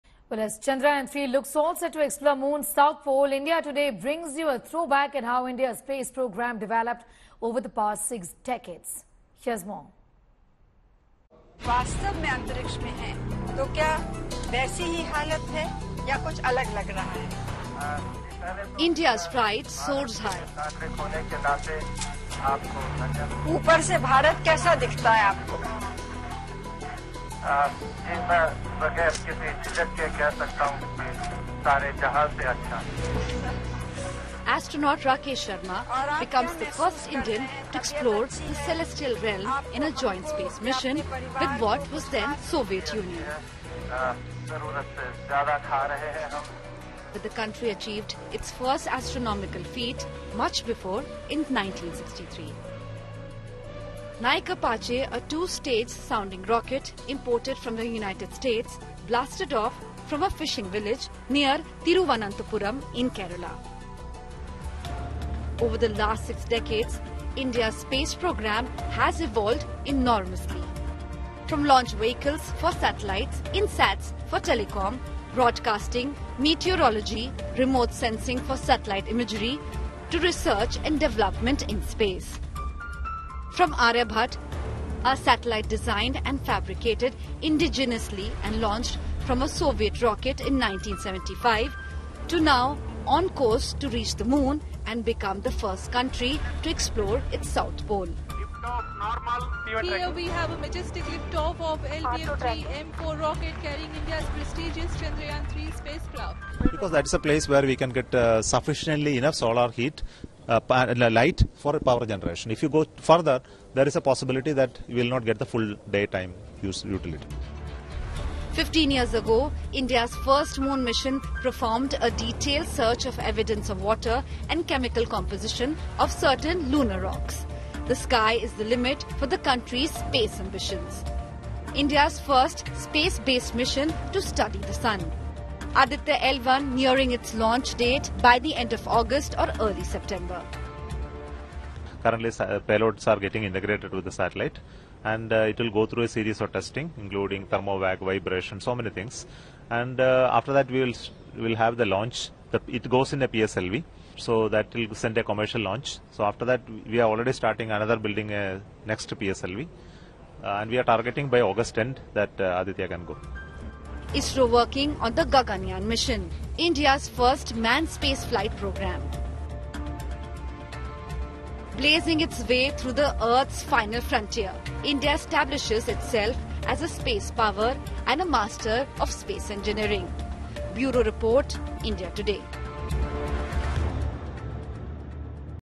Chandrayaan-3: Watch How India's Space Programme Developed Over The Past Six Decades
The Chandrayaan-3 mission is on schedule to attempt a soft landing on the Moon on Wednesday, August 23, the Indian Space Research Organisation (Isro) said. The space agency said all systems are undergoing regular checks and smooth sailing is continuing.